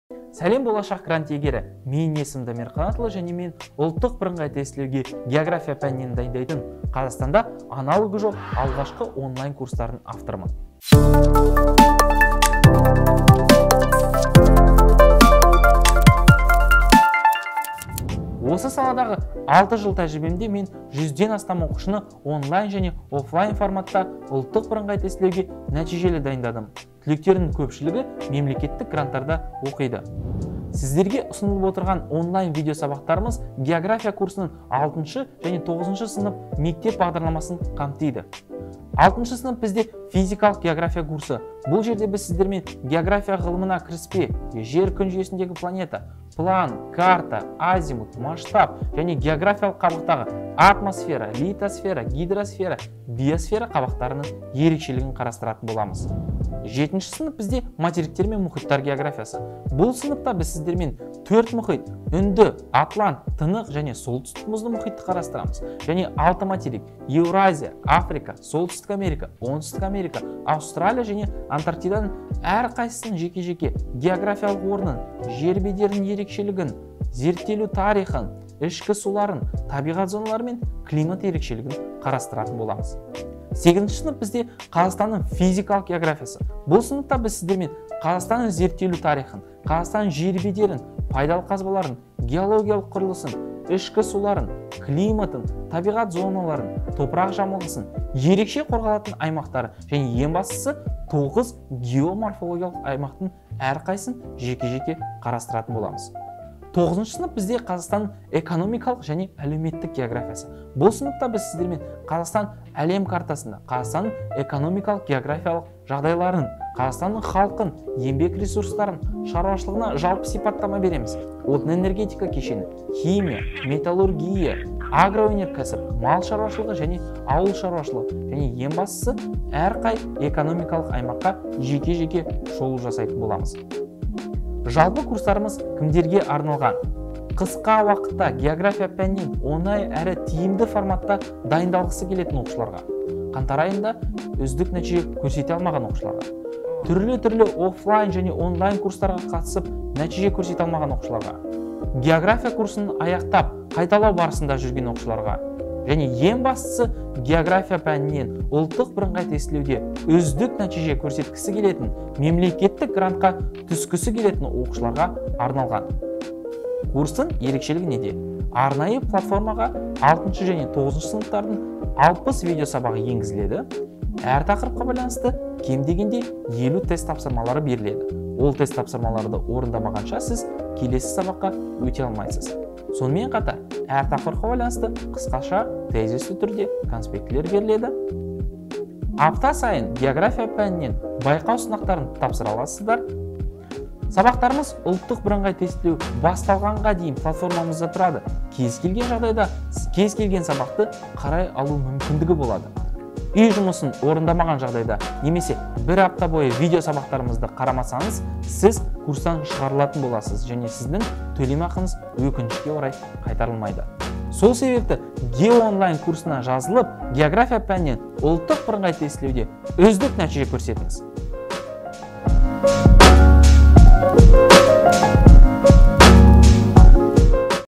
ГЕОГРАФИЯДАН ДАЙЫНДЫҚ КУРСТАРЫ ТУРАЛЫ АҚПАРАТ
Сәлем, жас түлек!
Сені, ҚАЗАҚСТАНДА аналогы жоқ, ГЕОГРАФИЯ пәнінен онлайн курстарға шақырамыз.
3-4 айдың ішінде кем дегенде 35+ ұпайға дейін деңгейіңізді көтереміз.